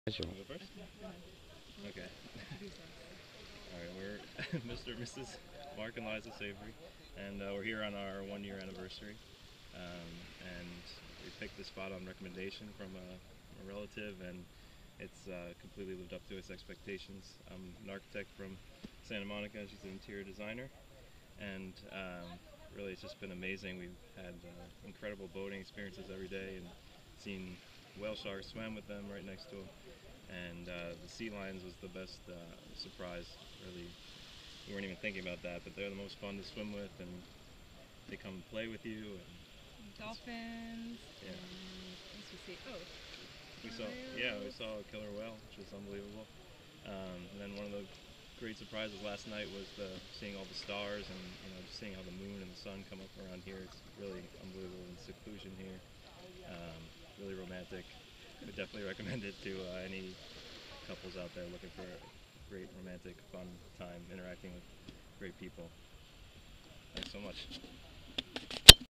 Baja Ecotourism and Sea of Cortez Adventure Travel with Baja Spirit - Happy Guests
Here are a young couple's quick comments that totally enjoyed staying at Las Animas Wilderness Retreat on the Sea of Cortez, Baja, Mexico

( formerly Baja AirVentures - Same company, owners and tours, just a new name)